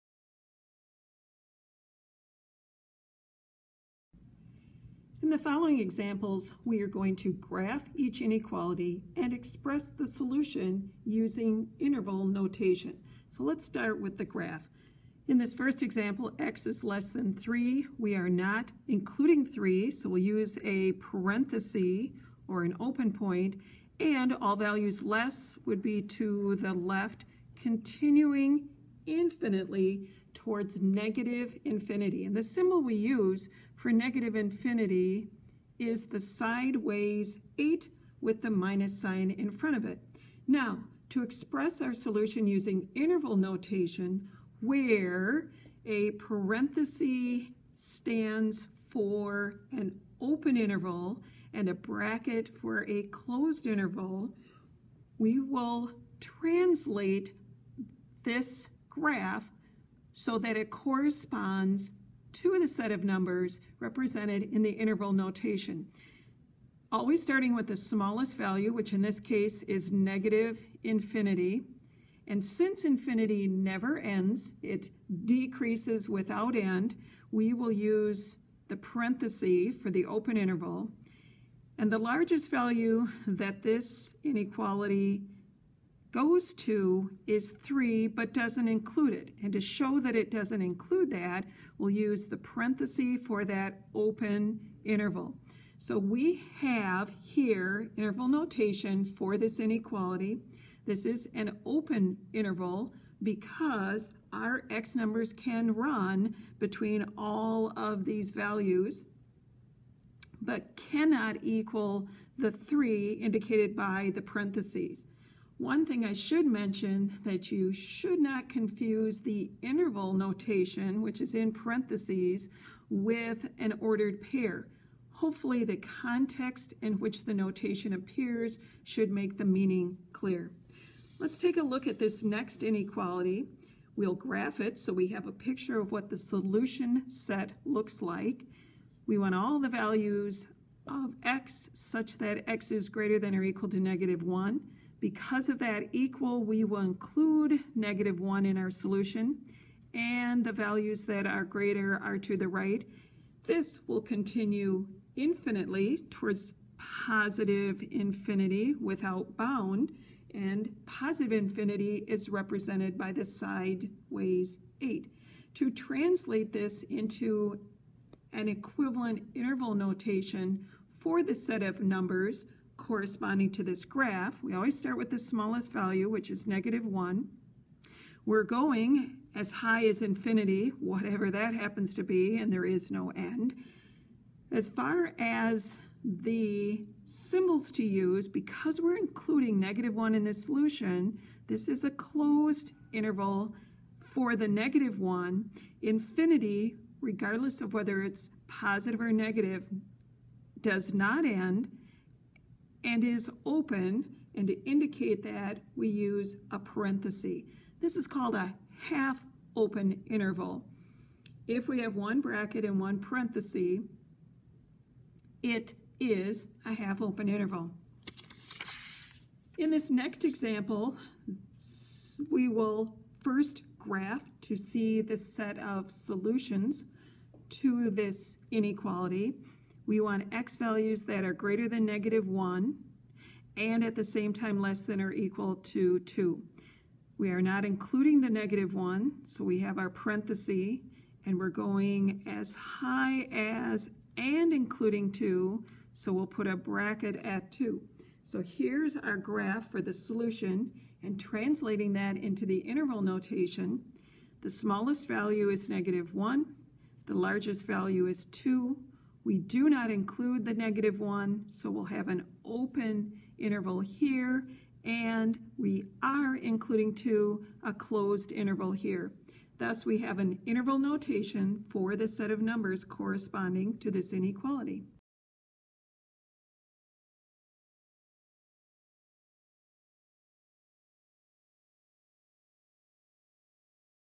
Solving Inequalities-Interval Notation – Elementary Algebra
Learn how to represent inequalities on a number line using interval notation.

This lecture video for Elementary Algebra is presented by Southwest Wisconsin Technical College mathematics instructor Helen Mar Adams.